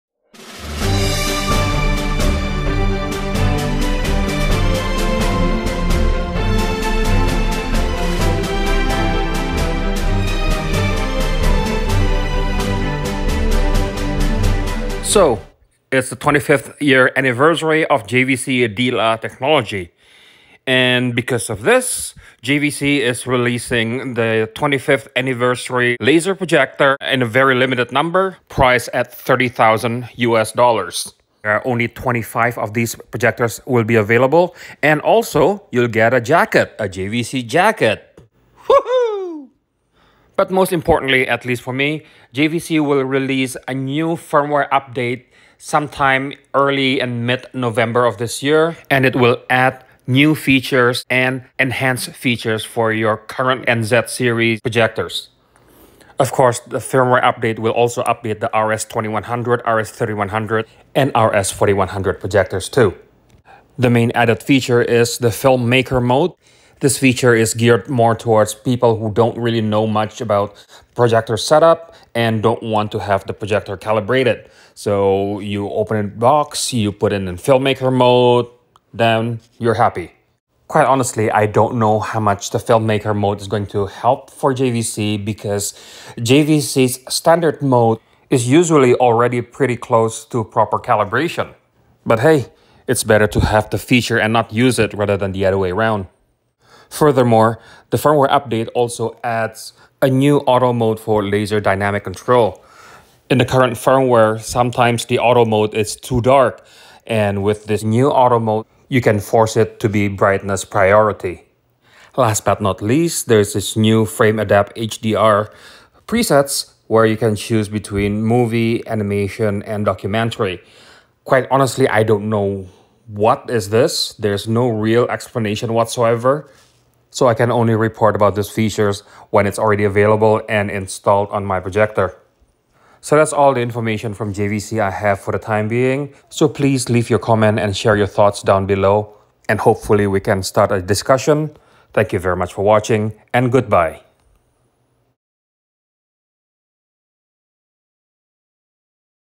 FREE new features from JVC 25th Anniversary of D-ILA
@CEDIAHQ Announcement of free new features added for @JVCglobal laser projectors

NZ7 NZ8 NZ9 RS2100 RS3100 RS4100